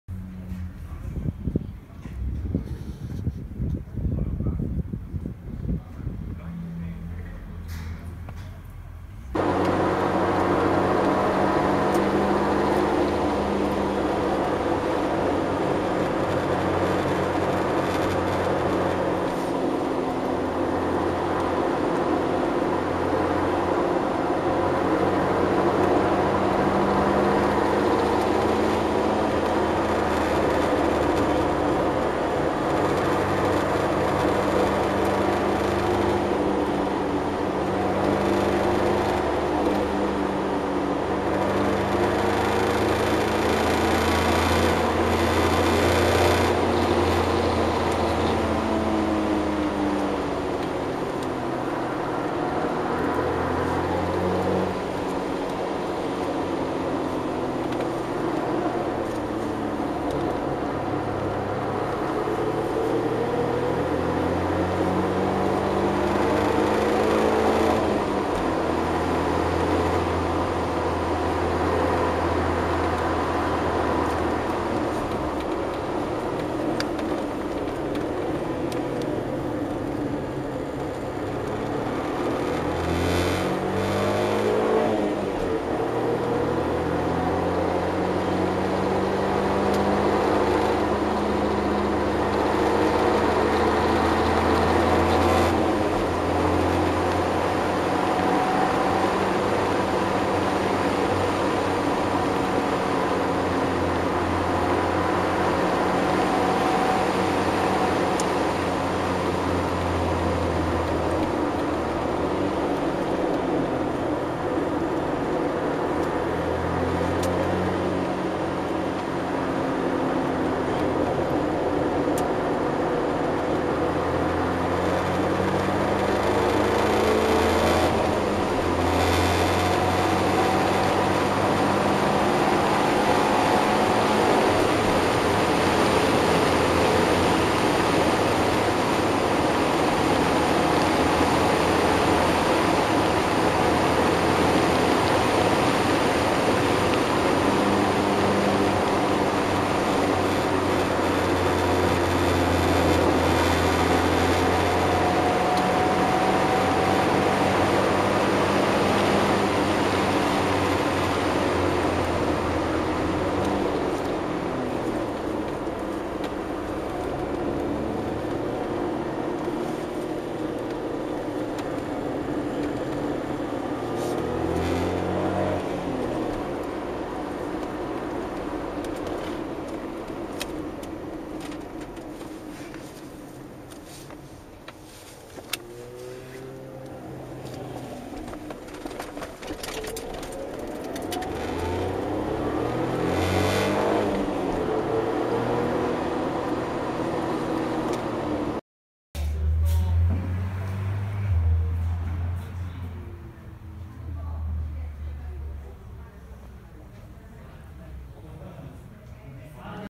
Suzuki Cappuccino(EA-11R F6A)Suzuka Circuit Test run Part⑤ Final February 25, 2021
We did a test run at Suzuka Circuit